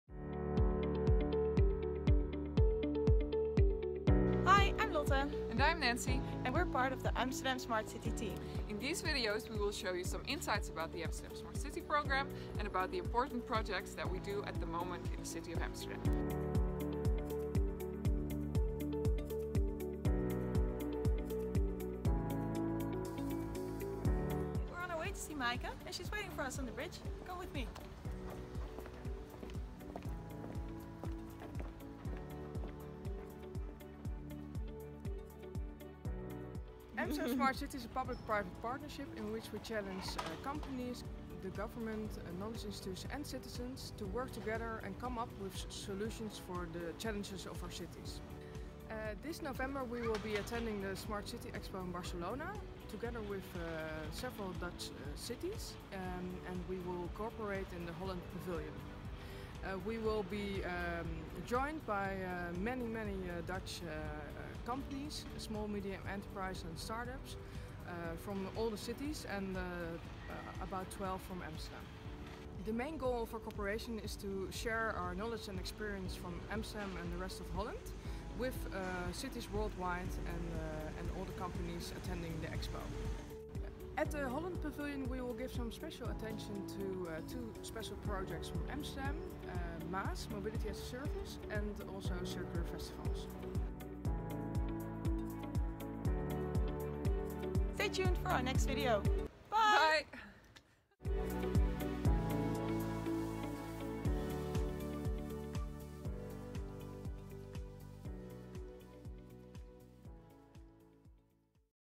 The road to Barcelona
Barcelona is an innovative city that hosts the Smart City Expo World Congress every year. In preparation of this event we have recorded this video. Enjoy!